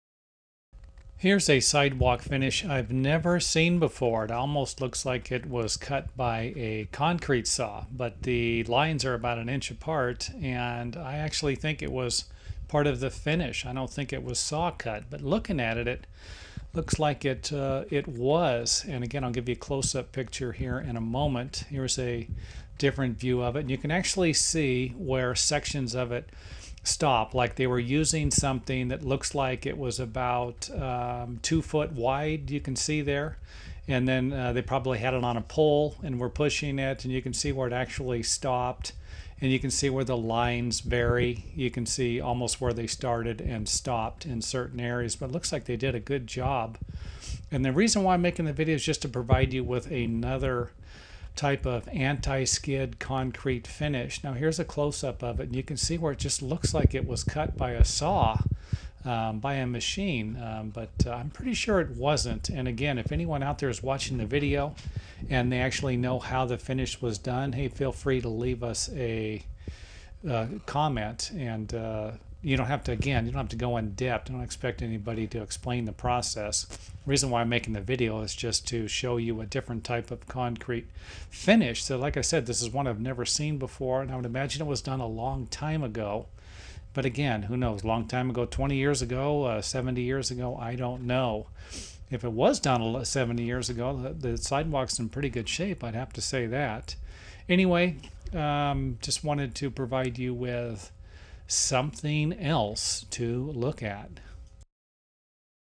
Check Out This Anti-Slip Sidewalk - Safety Engineering and Concrete Finishing
Click on this link for more information about sidewalks, building foundations and concrete. Realistically, it's hard for me to even tell how they finished this concrete, but it definitely falls into the category of antiskid and slip resistance walkways and in my book deserves an A+ on safety engineering and design. I think something like this would be hard to do, but like most things that are difficult, they're usually well worth it in the end. For more tips and interesting ideas about construction, building and sidewalks, visit our websites today.